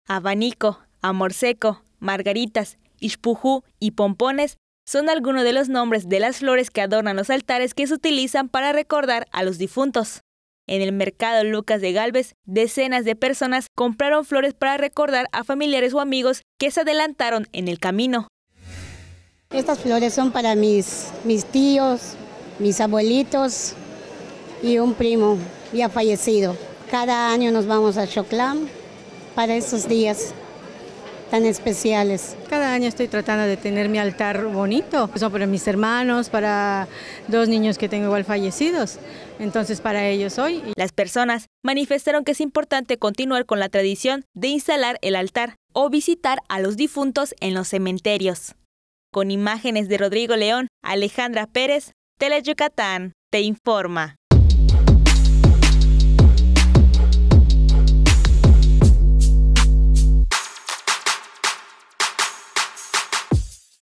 Opciones de flores que se usan en esta temporada de Fieles Difuntos | TELE Yucatán
Estas son todas las opciones de flores que se usan en esta temporada de Fieles Difuntos.